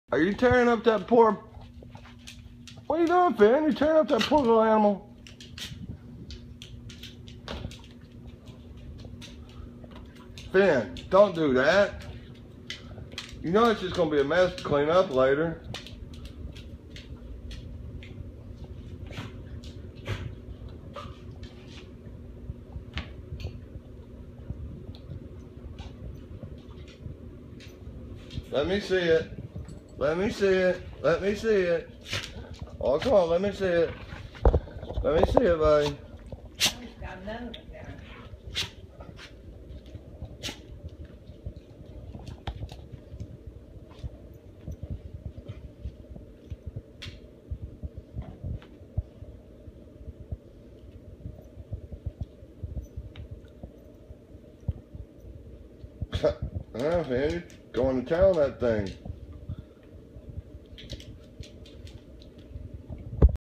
Wolf Dog at play funny
this is the hottest best video i have

My wolf hybrid tearing up another toy. He is so gunny to watch he is almost like a human being you just have to watch.